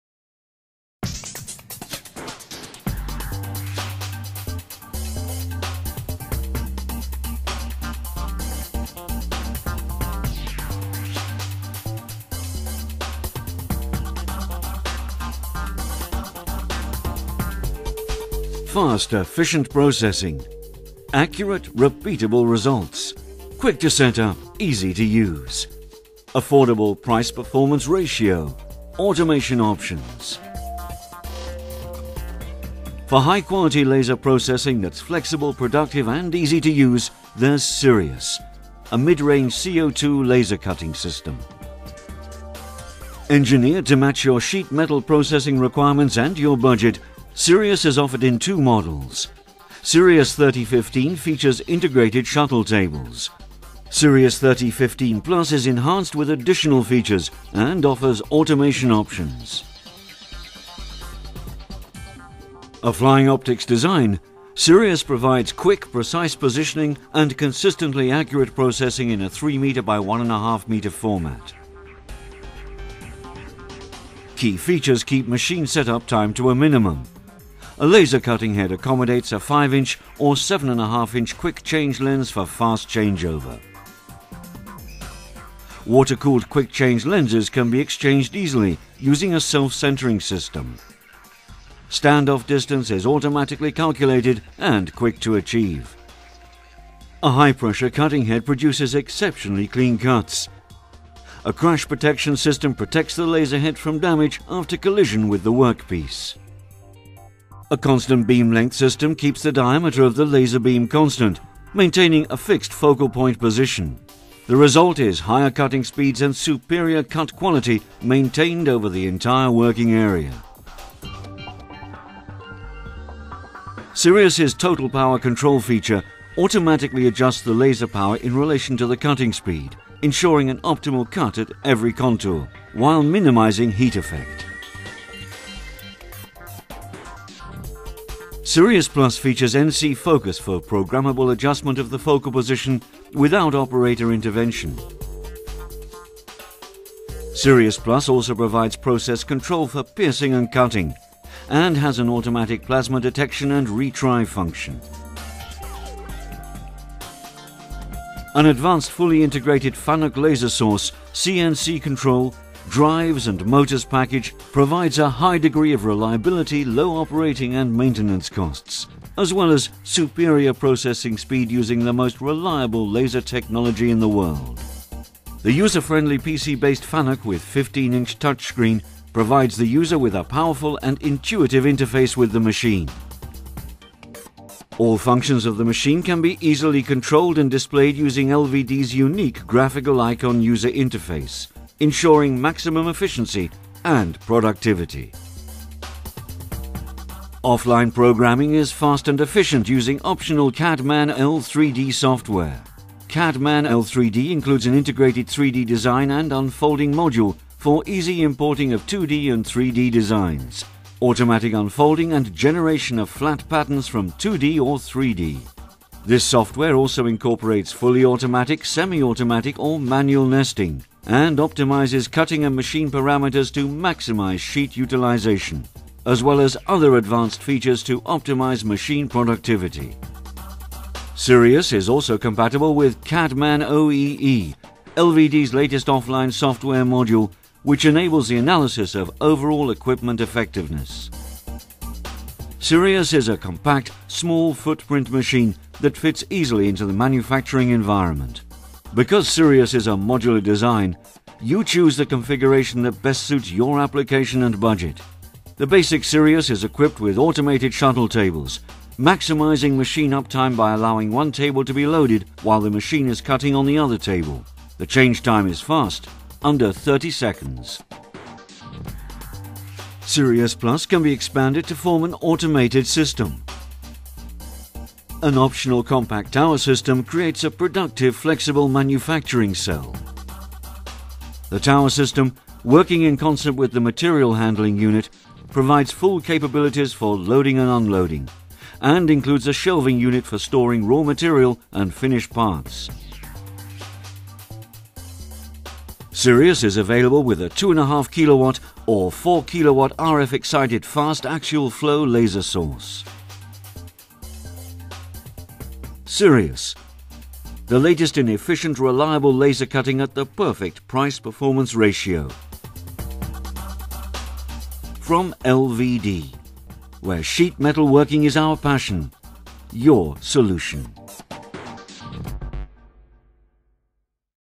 LVD's Sirius Laser Cutting Machine with CT-L
MID-LEVEL LASER CUTTING SYSTEM FOR FAST, EFFICIENT SHEET METAL PROCESSING

- Efficient processing of parts at optimal speeds and accelerations to
  suit the part geometry
- Reliable cutting performance at an affordable price-performance ratio
- 1500 x 3000 mm (5' x 10') work table area
- Incorporates automatic shuttle tables
- Quick change lens (5" and 7.5")
- Capacitive height sensing maintains a constant distance between
  the head and the material being processed and adjusts to any
  undulations in the plate
- Constant beam length system ensures consistent cutting over the
  entire cutting area
- NC Optimization maximizes machine productivity without operator
  intervention by automatically determining the cutting head motion
  based on travel distance
- Fast cut-to-cut workpiece changeover
- High reliability with low maintenance and operation costs
- Integrated LVD - Fanuc machine, control, laser source and motor
  drive package
- Feature enhanced Sirius Plus model offers affordable automation
  possibilities

Key Options:
- Loading arm
- Choice of 2.5 or 4 kW laser sources
- CADMAN-L 3D offline software
- Sirius Plus includes additional functionality to allow the addition of
  automation
- Compact Tower (CT) 4, 6 or 10 pallet automated material handling
  system (on Sirius 3015 Plus)

Models available:
Sirius 3015
Sirius 3015 Plus